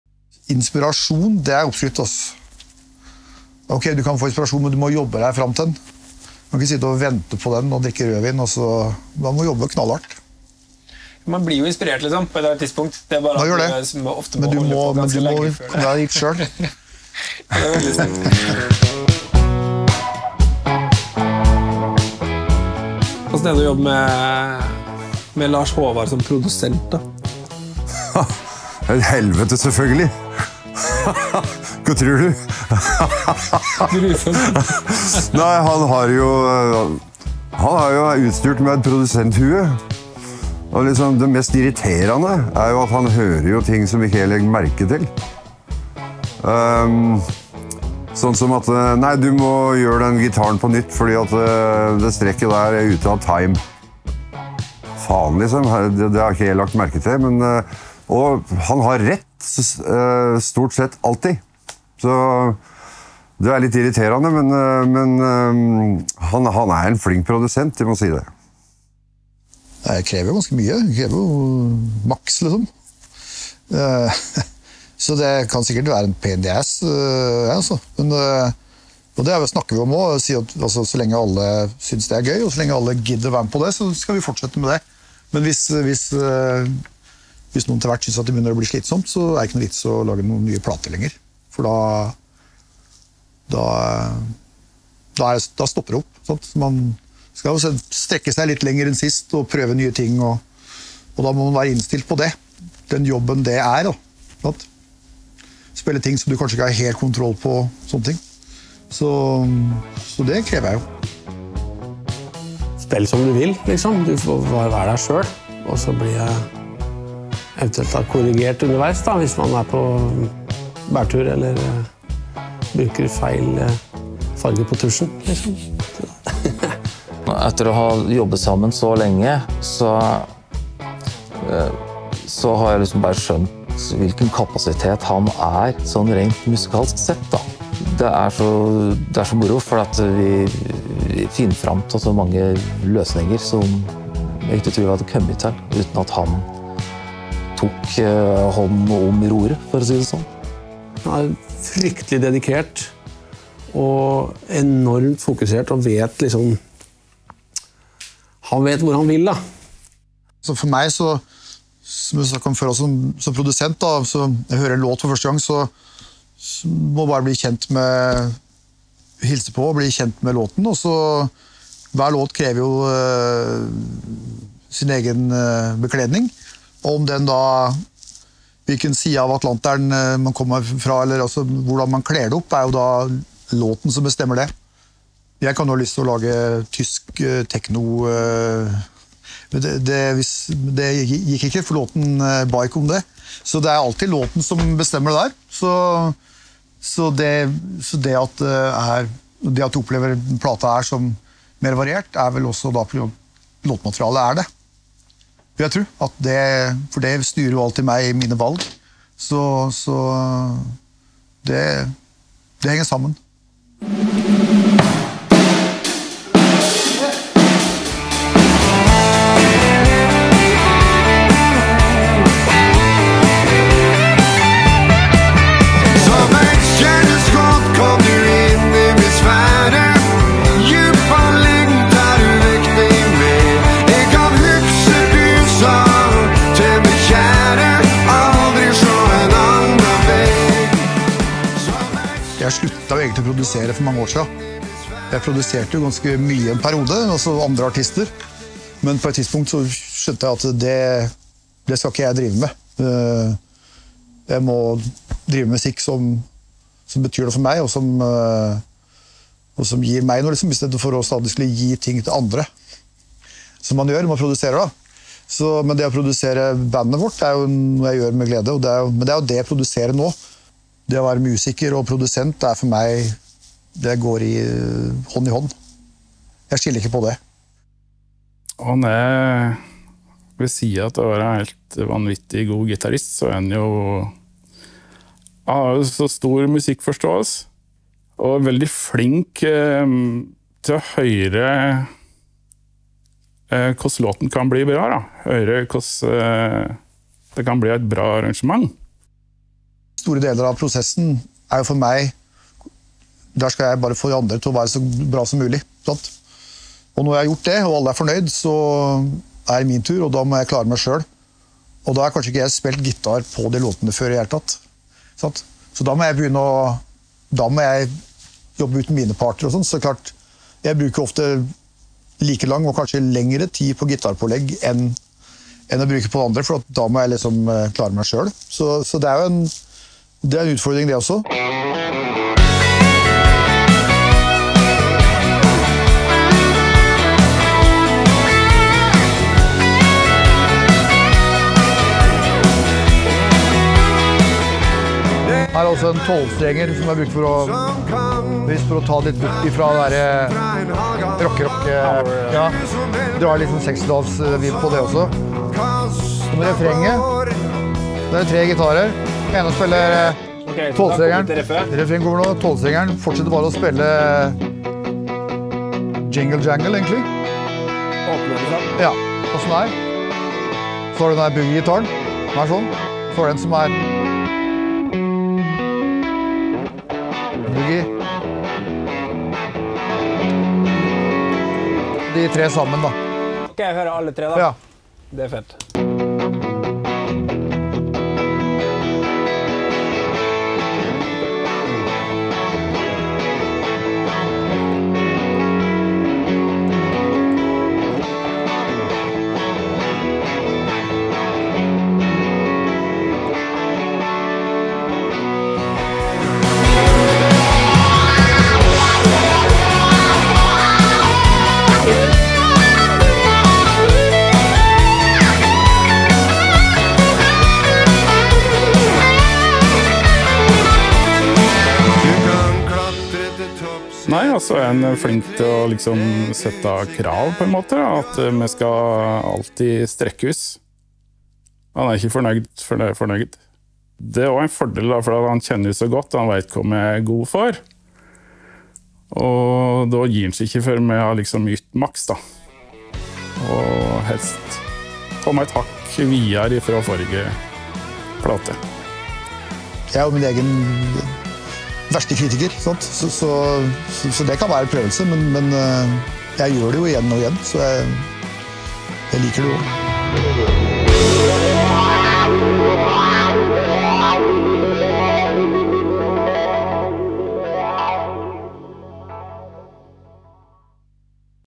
Hellbillies - produsent Lars Haavard Haugen (ep. 4:6)
Lars Haavard Haugen i Hellbillies har produsert alle bandets album siden 2004, og har i stor grad bidratt til hvorfor Hellbillies låter som det gjør idag.
I denne episoden får vi et innblikk i arbeidet hans som produsent, men også hvilke utfordringer det byr på.

Med:
Lars Haavard Haugen, Aslag Haugen, Bjørn Gunnar Sando, Egil Stemkens og Lars Christian Narum.
Intervjuer: Øyvind Blomstrøm
Film & foto: Nikolai Grasaasen, Anders Lindstad, Egil Eliassen
Klipp: Nikolai Grassasen
Lyd: Christer Krogh